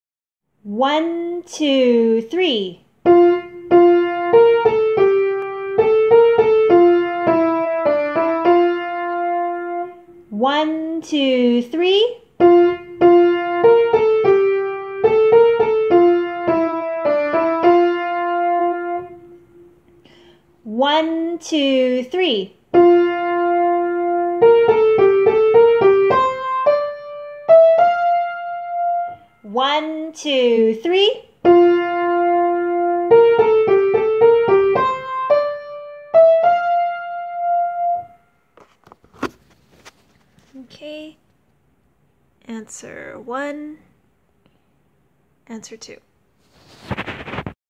RCM 2015 Level 3 Rhythm Clapback - 3/4 time, exercise A
Exercises are played twice. After the second time, pause the video and clap/tap/hum back the rhythm. Answer are at the end of the video.

Exercise 1: start the video
Exercise 2: